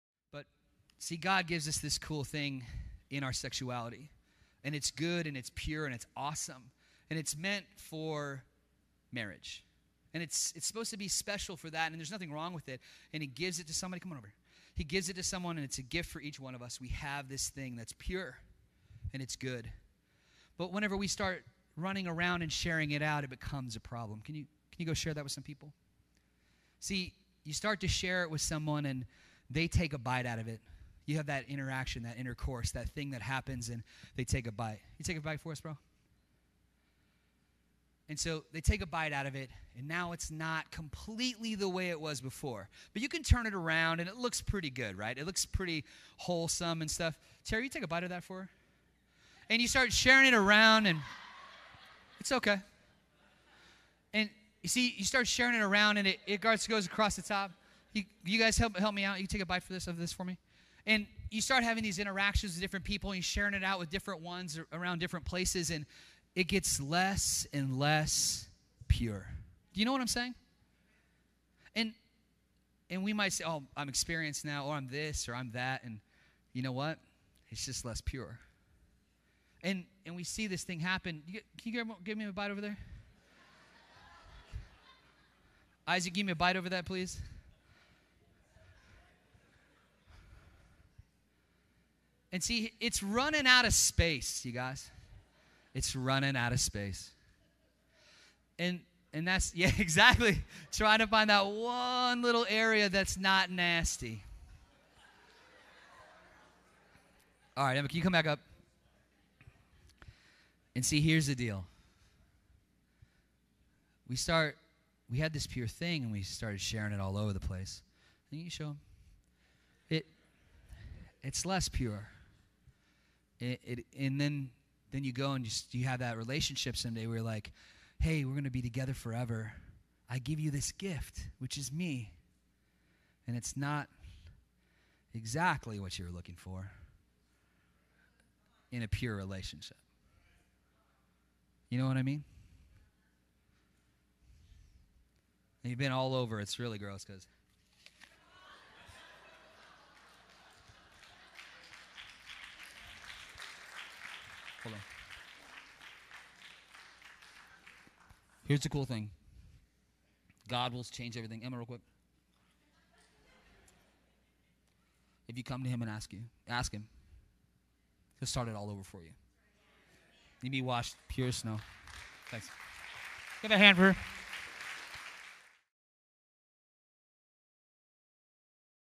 Cornerstoneaz org | 11 17 19 | Surviving the 7 Deadly Sins | The Apple Example
Pastor J shares the apple example about the spiritual effects of sexual promiscuity.